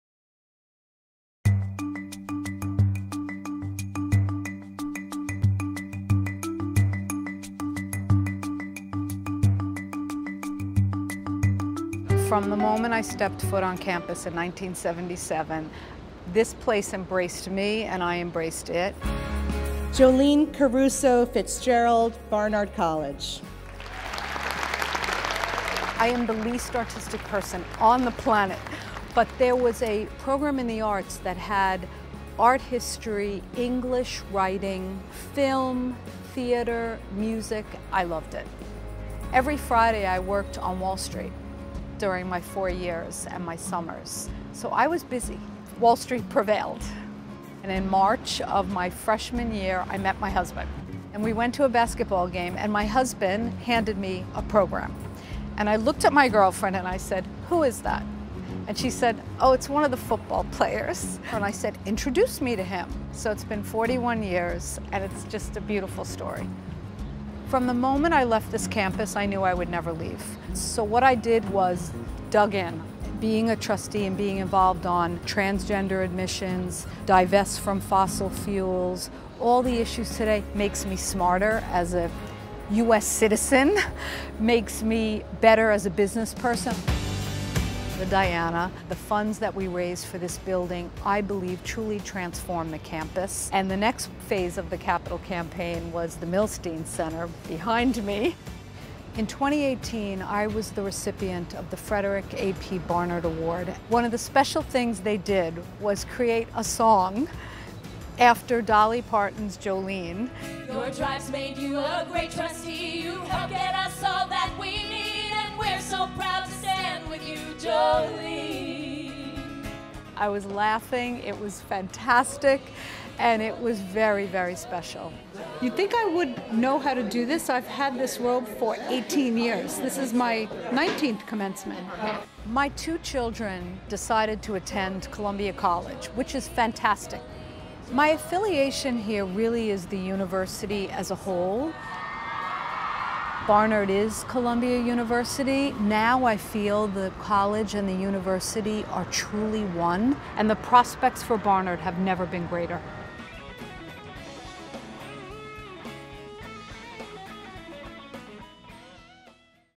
2019 Alumni Medalist Jolyne Caruso-FitzGerald '81BC
Jolyne Caruso-FitzGerald '81BC is founder and CEO of The Alberleen Group, an investment advisory firm that works with global family office investors.  Throughout her career, Caruso-FitzGerald has held senior roles at major investment banks, including JP Morgan and Lehman Brothers, where she helped build and manage global capital markets and alternative asset management businesses. She is chair emerita of Barnard College’s Board of Trustees. She has been a trustee since 2000 and was board chair from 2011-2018. She was the 2018 recipient of The Frederick A.P. Barnard Award.

Highlights of her board tenure include overseeing the first major capital campaign in Barnard’s history, which included the building of the Milstein Teaching and Learning Center; the landmark decision to admit transgender students to Barnard, as well as chairing and serving on the presidential search committee for Barnard’s last two presidents. Caruso-FitzGerald has served on the advisory board of Columbia Entrepreneurship since 2015 as well as on numerous non-profit and private company boards. Her husband Shawn FitzGerald ’80CC and their children, Christian FitzGerald ’17CC and Gabrielle FitzGerald ’19CC are all Columbia College graduates.